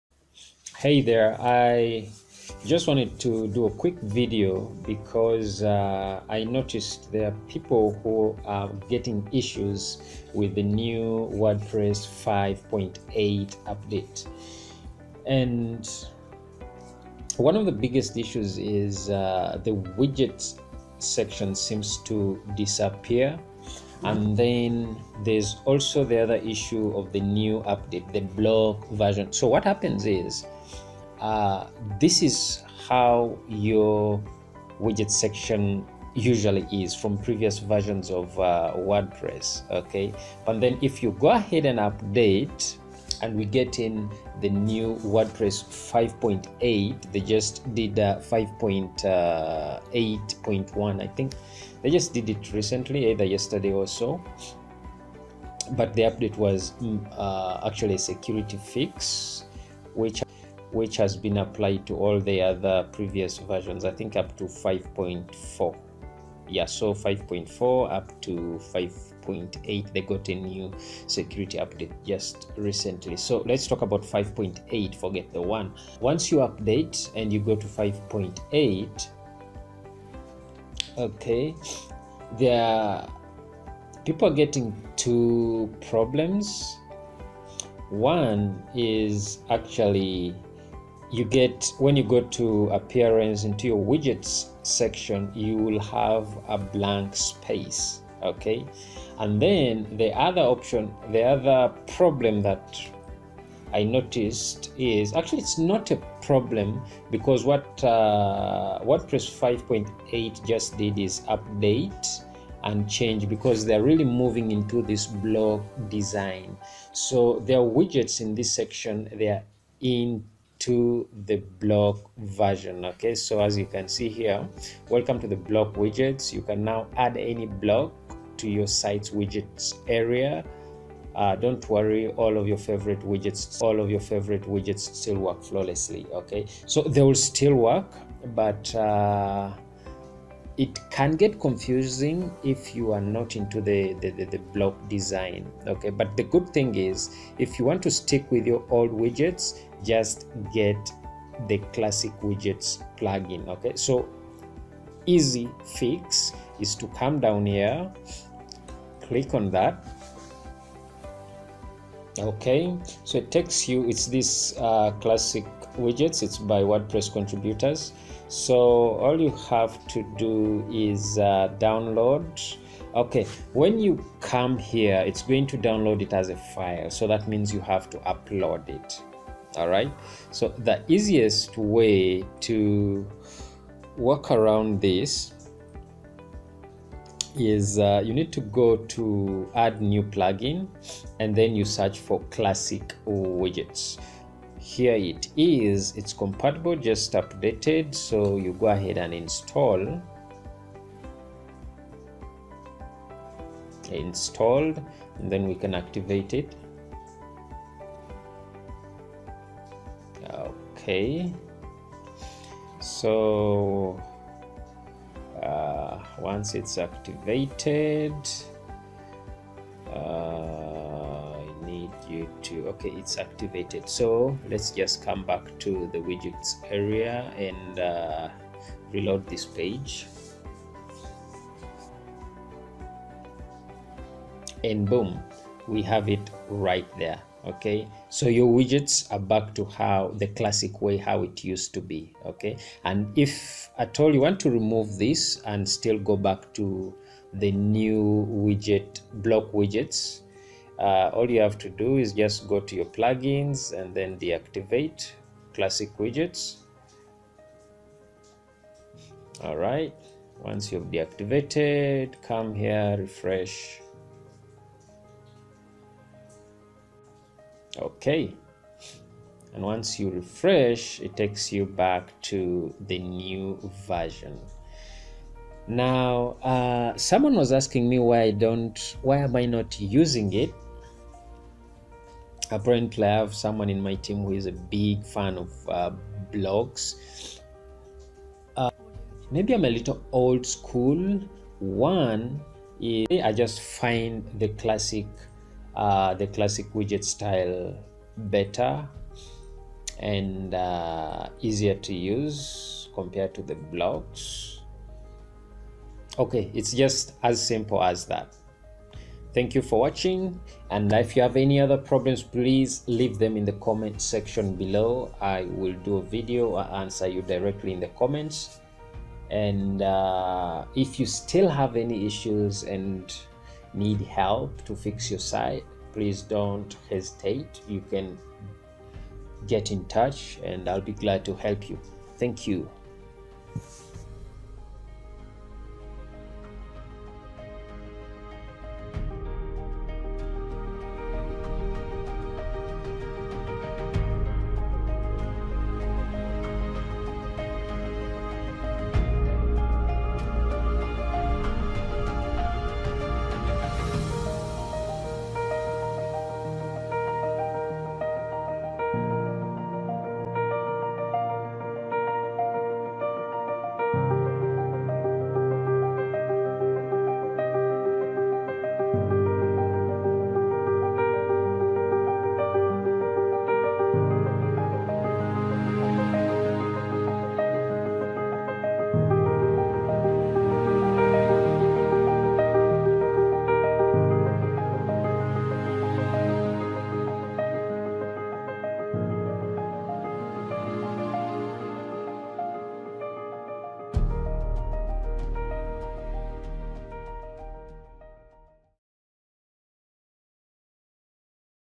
How to Fix WordPress Widget not Working or Loading (FIXED)
WordPress Widget area showing blank page. A Fix to WordPress Widget not loading or Loading.

In the most recent new WordPress updates, many WordPress users have been faced with the problem of the WordPress Widget not working. Sometimes the WordPress Widget is not loading properly.

So If you're wondering why your WordPress Widget is not working, then this video is to help you Fix the WordPress Widget problem. This problem has been rampant with the New WordPress 6.8 Update version.

WordPress 6.8 has introduced Block Widgets so if you're happy with the Block text Editor, I'm sure you will like this new WordPress Block Widget section. But if you're like me, and you like the classic editor, then this video will help you revert back from the New Block Widget to the old WordPress Widget. And it will also fix any Widget not loading problem.